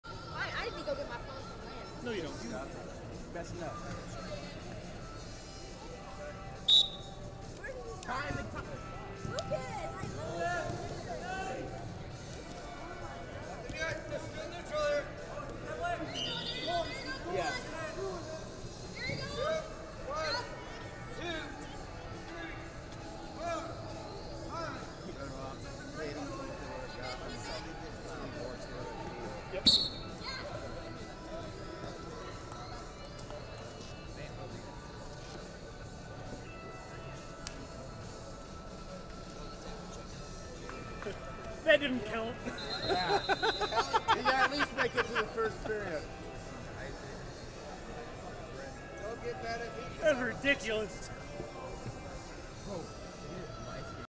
119 Lbs Cons. Round 3 - Owen Schacht, X-Factor Elite Wrestling Vs Aidan Deal, Wisconsin Ebe7
FloSports/2022 Folkstyle Tour Of America Dominate In The Dells & Showcase Qualifier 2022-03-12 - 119 Lbs Cons. Round 3 - Owen Schacht, X-Factor Elite Wrestling Vs Aidan Deal, Wisconsin Ebe7